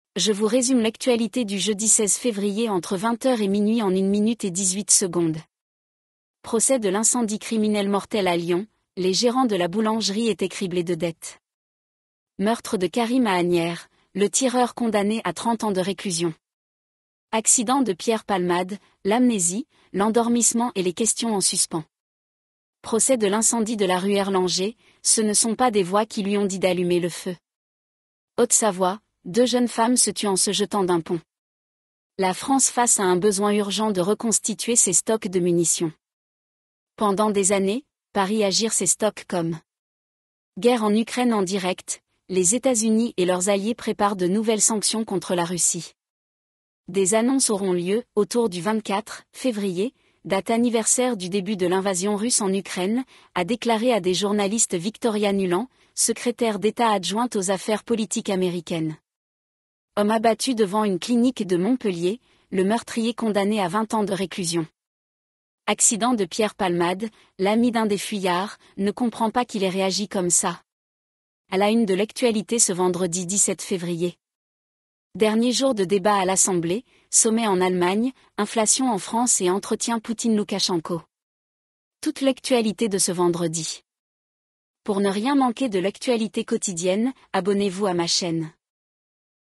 Résumé de l'actualité du jour : Jeudi 16 février entre 20h et minuit
- Procès de l’incendie criminel mortel à Lyon : les gérants de la boulangerie étaient criblés de dettes
- Meurtre de Karim à Asnières : le tireur condamné à 30 ans de réclusion
- Accident de Pierre Palmade : l’amnésie, l’endormissement et les questions en suspens
- Procès de l’incendie de la rue Erlanger : « Ce ne sont pas des voix qui lui ont dit d’allumer le feu »
- Haute-Savoie : deux jeunes femmes se tuent en se jetant d’un pont
- La France face à un besoin urgent de reconstituer ses stocks de munitions
- Guerre en Ukraine en direct : les Etats-Unis et leurs alliés préparent de nouvelles sanctions contre la Russie
- Homme abattu devant une clinique de Montpellier : le meurtrier condamné à 20 ans de réclusion
- Accident de Pierre Palmade : l’amie d’un des fuyards « ne comprend pas qu’il ait réagi comme ça »
- À la une de l'actualité ce vendredi 17 février...
- A Bobigny, un meeting contre la réforme des retraites pour les insoumis à Mélenchon